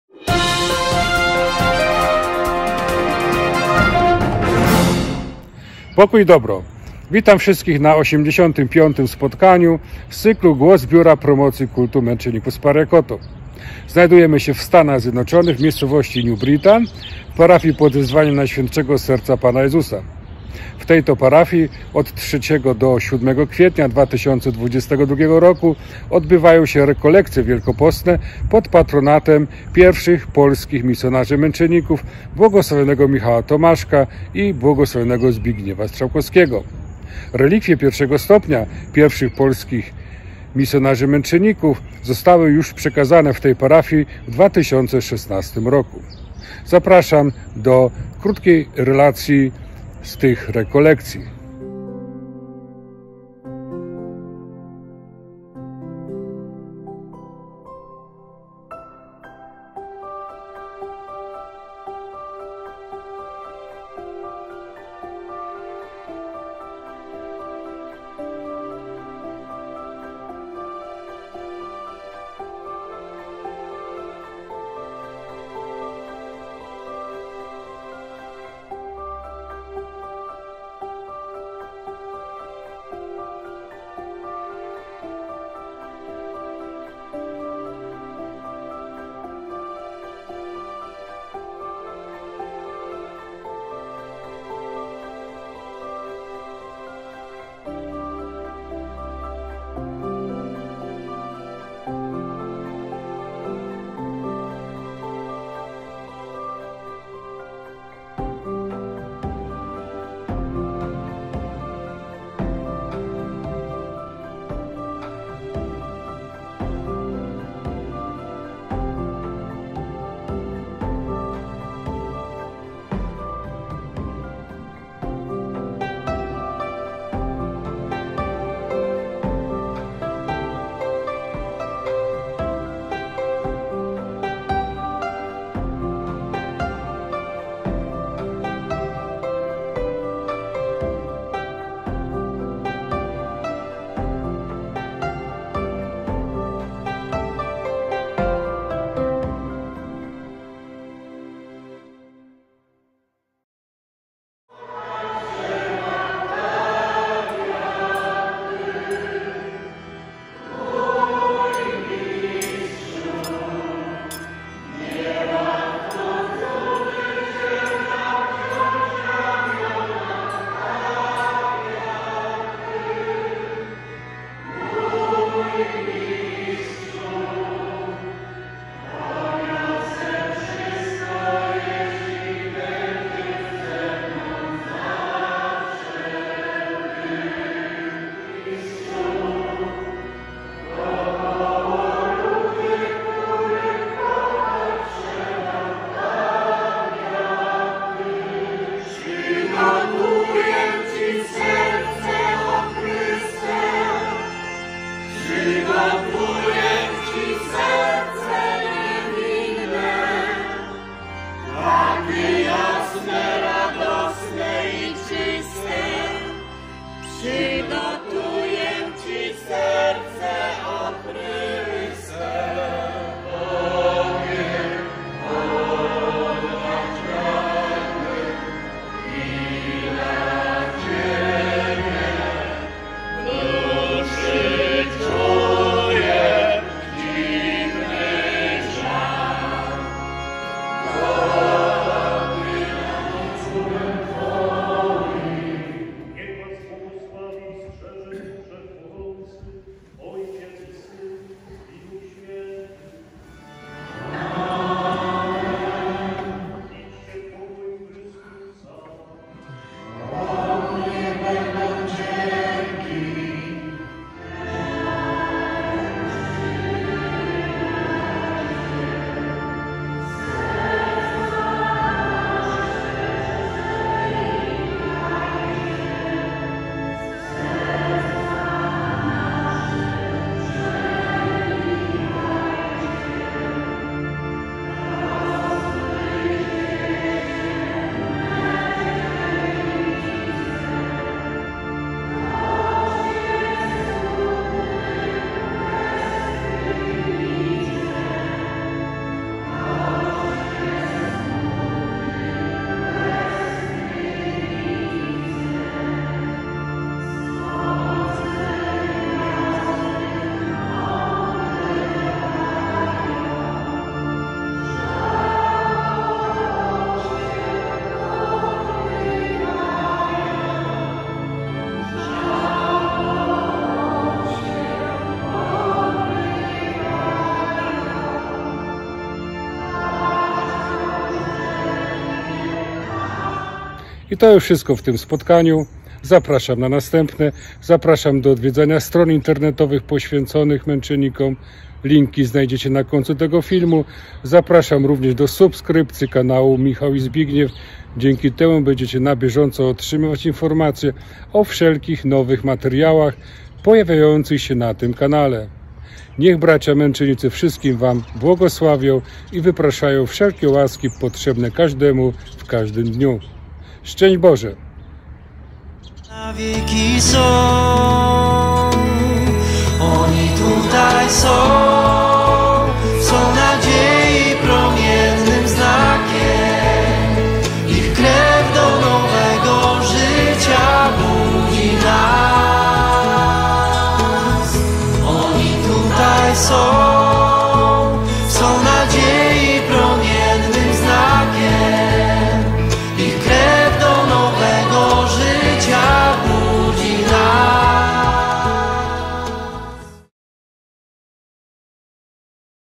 USA - NEW BRITAIN, CT- Rekolekcje Wielkopostne - 05.04.2022r.- Głos z BPKMzP - odc. 85
USA - NEW BRITAIN, CT- Parafia pw. Najświętszego Serca Pana Jezusa  - rekolekcje Wielkopostne z Męczennikami z Pariacoto -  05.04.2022r.- Głos z Biura Promocji Kultu Męczenników z Pariacoto - odc. 85.